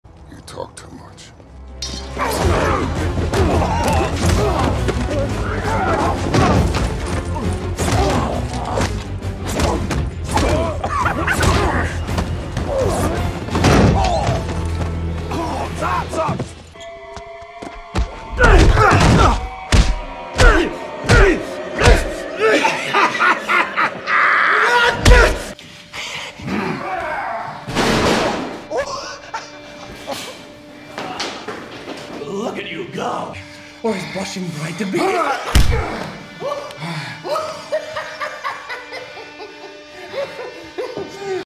Movie Villains Laughing As They Get Beat Up
Look at you go.